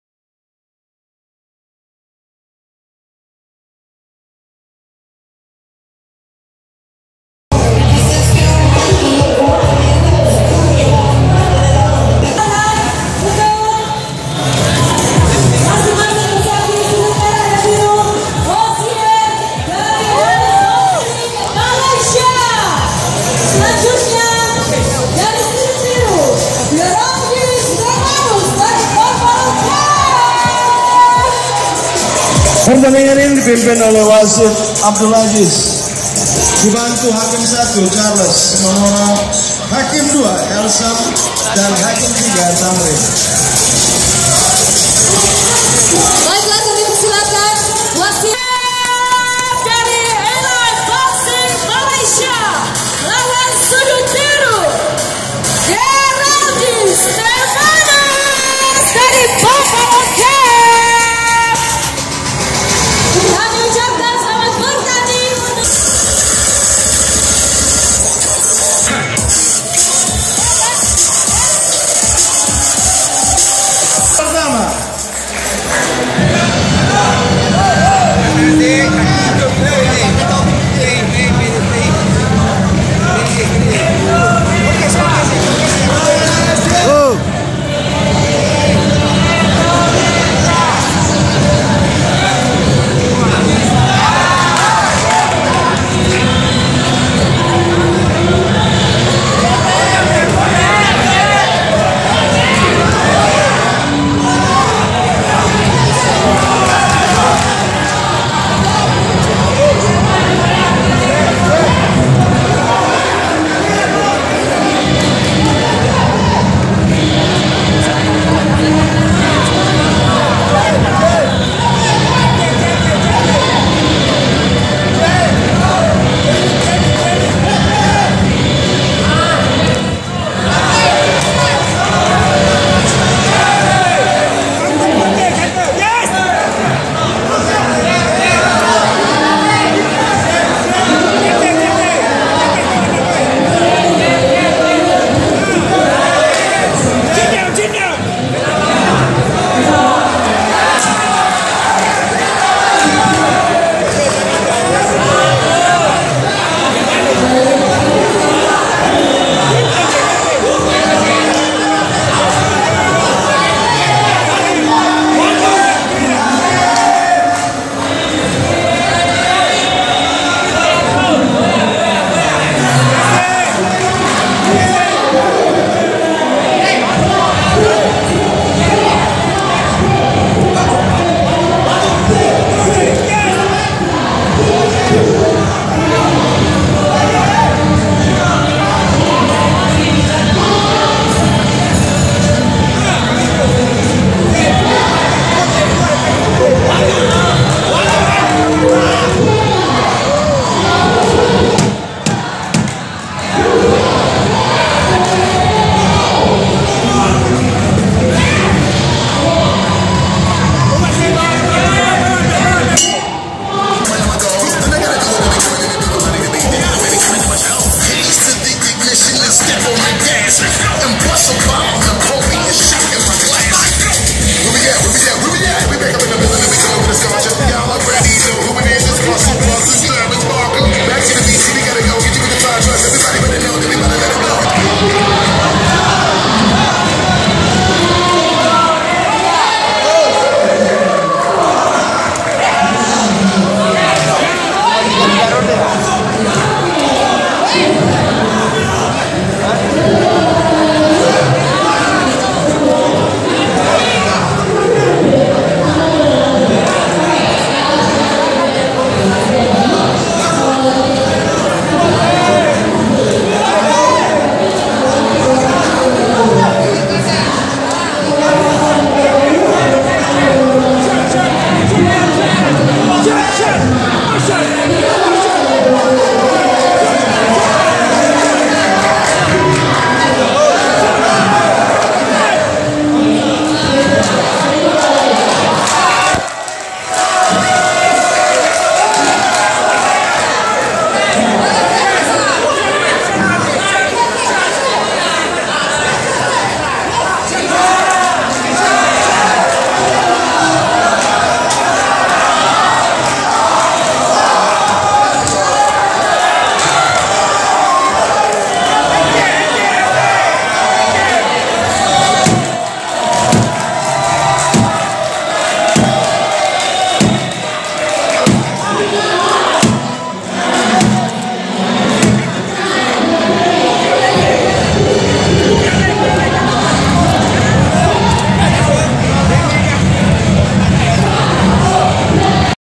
MUHAMMAD OZIER(Elite Boxing Malaysia) Vs GERALDI STEFANUS (Buffalo Camp)
Rookie Amateur debut boxers Muhd.Ozier dari @elite.boxing.club Malaysia.